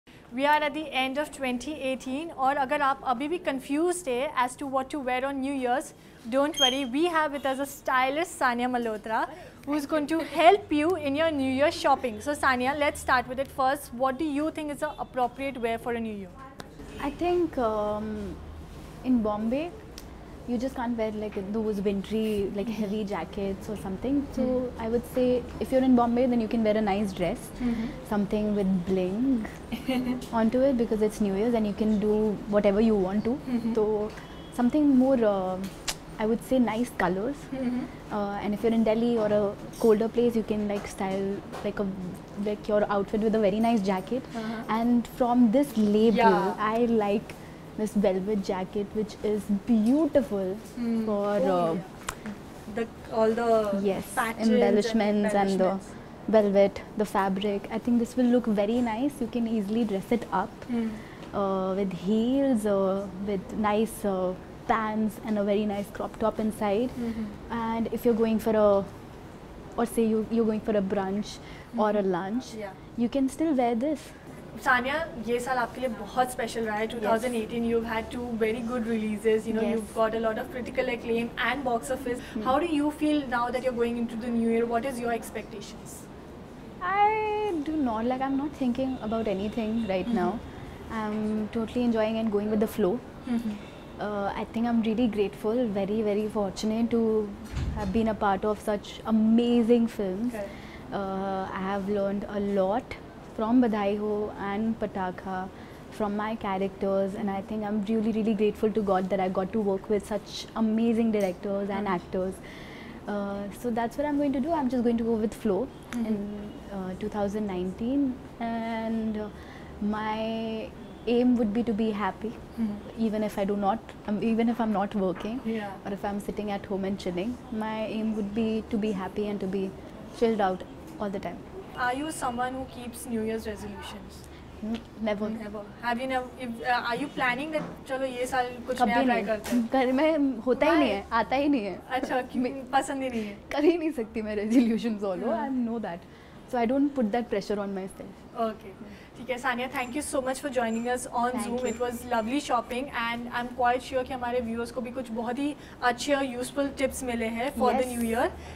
Sanya Malhotra turns your personal New Year's party stylist on Zoom | Exclusive
Sanya Malhotra shares her fashion tips and turns your personal New year's party stylist in an exclusive interaction with Zoom. Watch the video to know more.